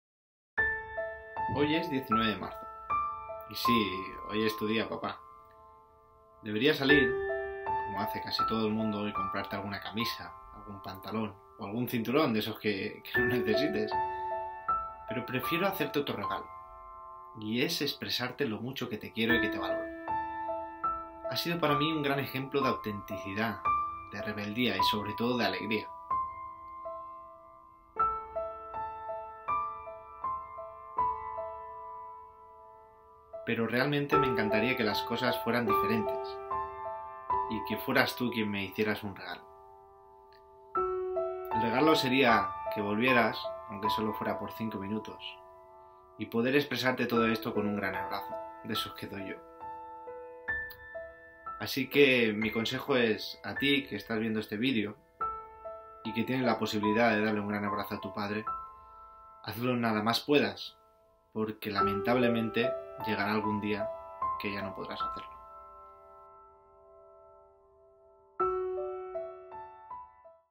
DÍA DEL PADRE | El mejor regalo es un mensaje
Un padre es un maestro, un regalo, una estrella. Está para guiarte y protegerte aunque muchas veces no sabe muy bien cómo hacerlo. Te pido que le hagas el mejor regalo posible, porque habrá algún día que desafortunadamente no estará para que se lo entregues.